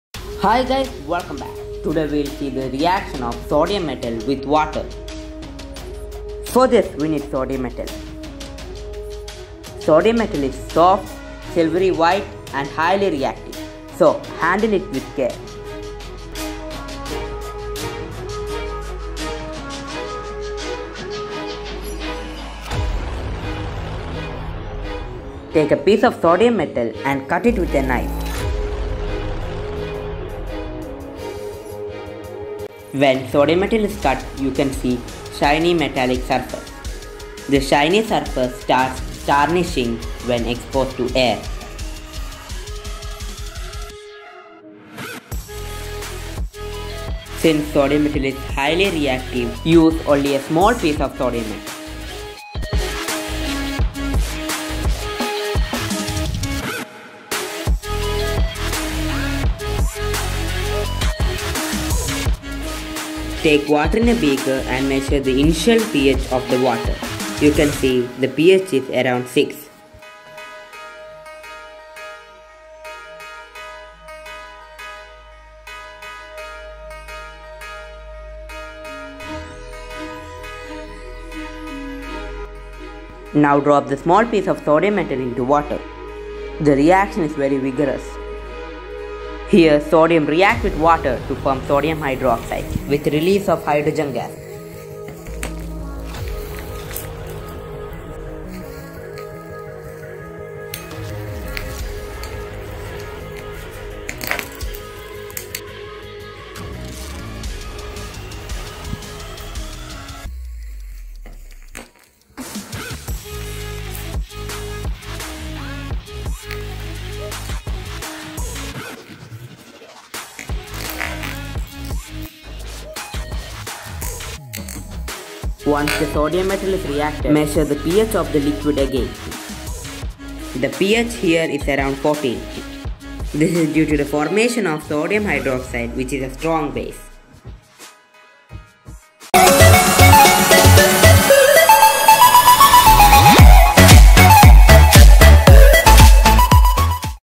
Reaction of sodium metal with water
In this video i am gonna saw you the reaction of sodium metal with water